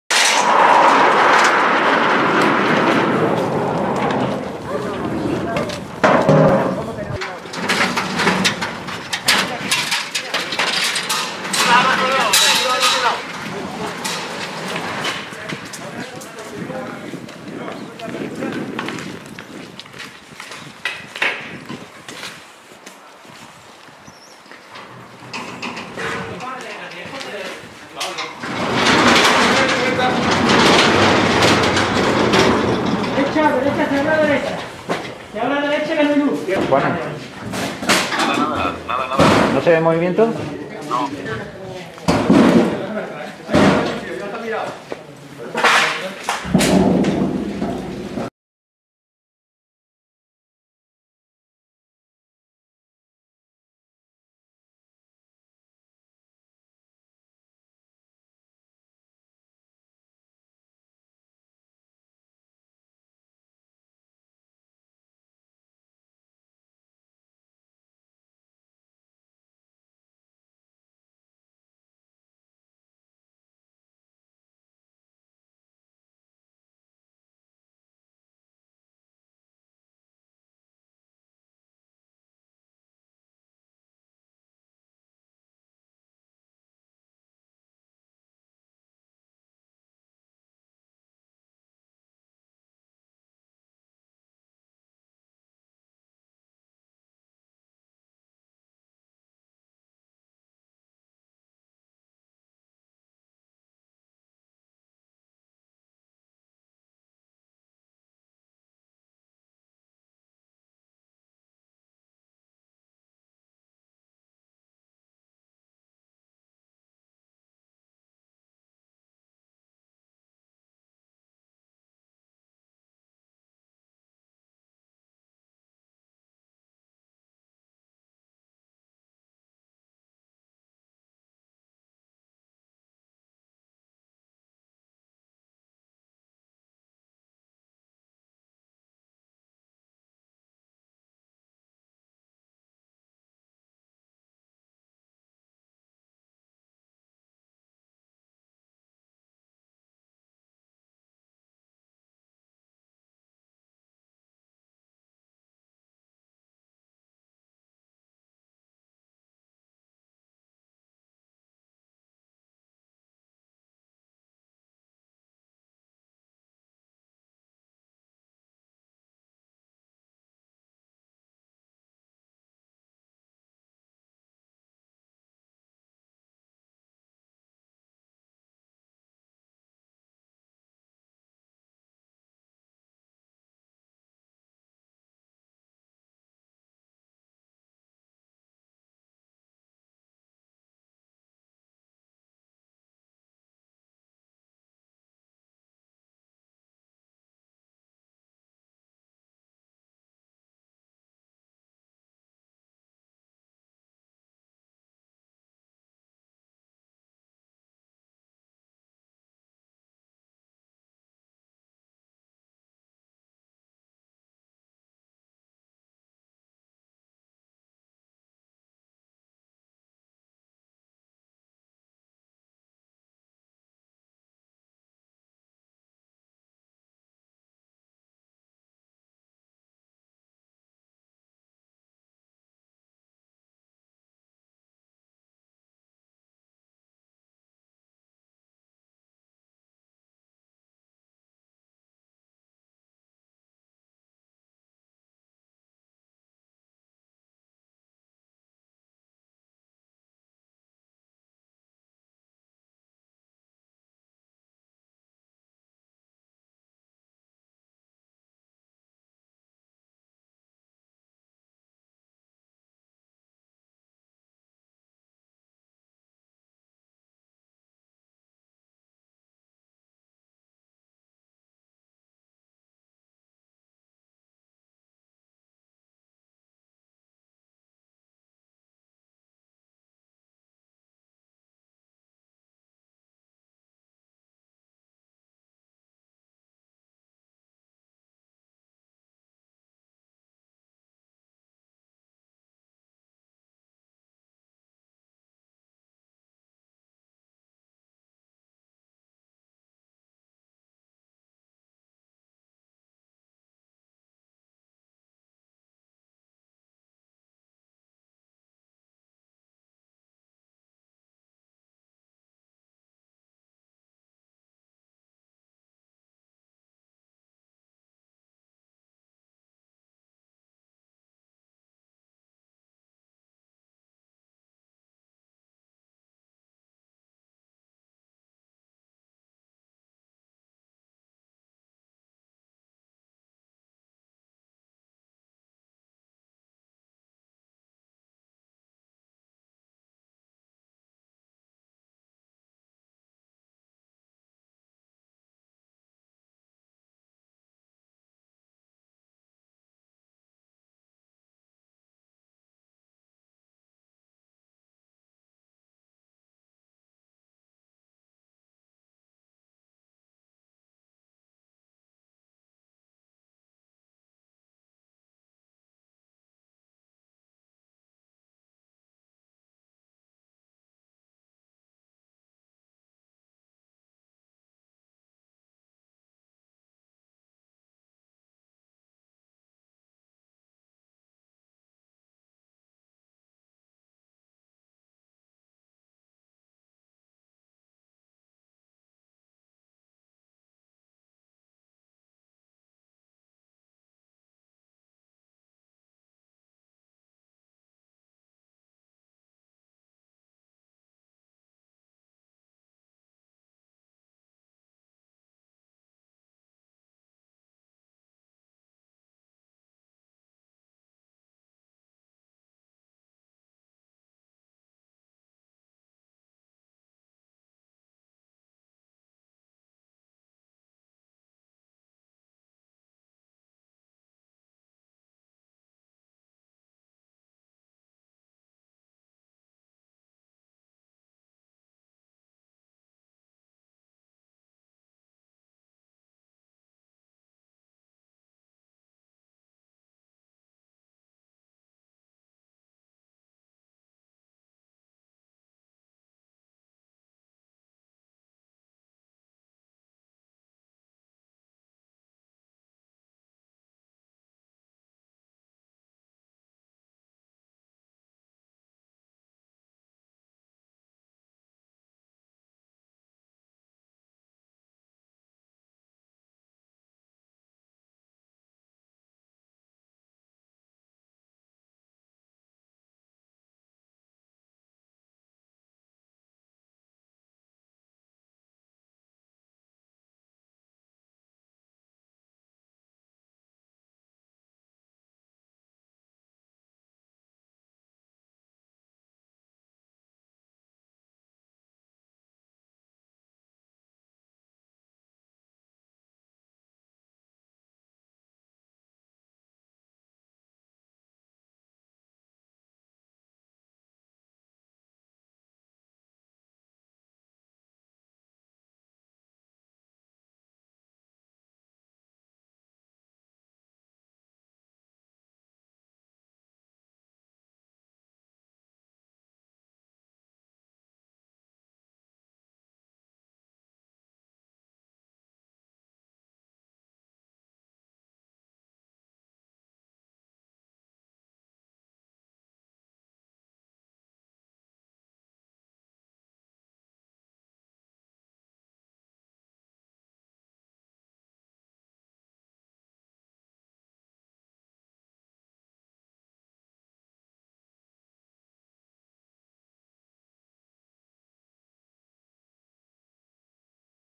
Gran red de producción de tabaco y marihuana ilegal a nivel europeo
Las autoridades españolas desmantelan una red criminal internacional con capacidad para producir toneladas de tabaco ilegal y marihuana para clientes de toda Europa